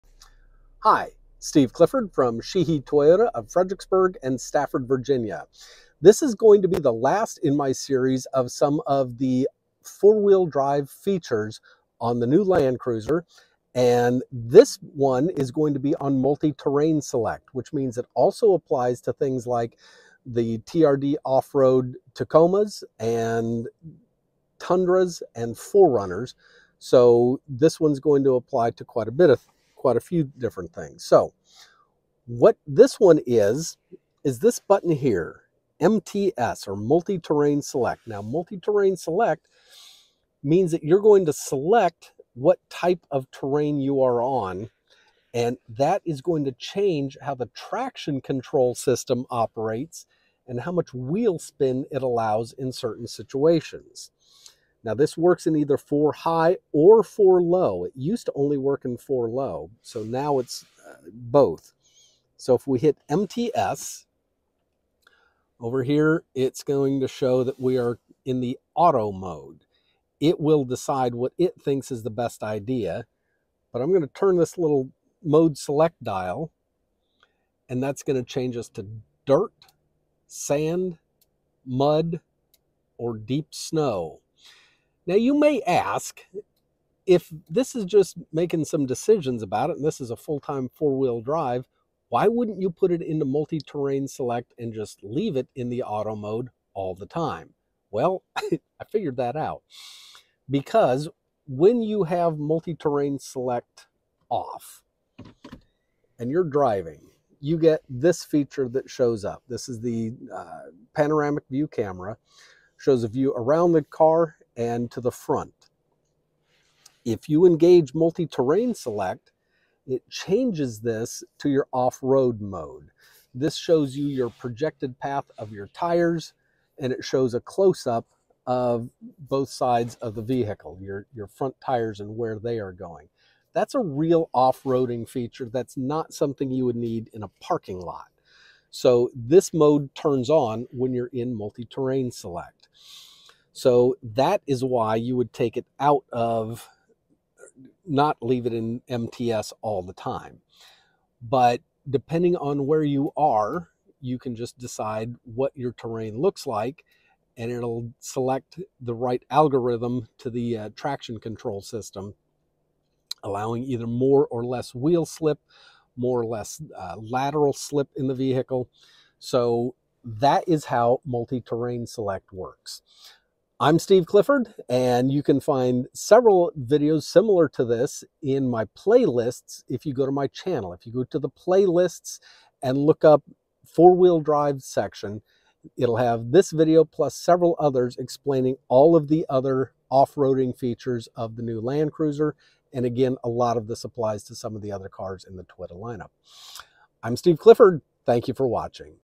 Toyota multi-terrain select
MTS allow the driver to tell the truck what kind of terrain it is in and it changes the traction control algorithm to maximize performance. This feature is available on the Tundra, Tacoma, 4Runner and Land Cruiser and to a lesser degree, the RAV4 and Highlander.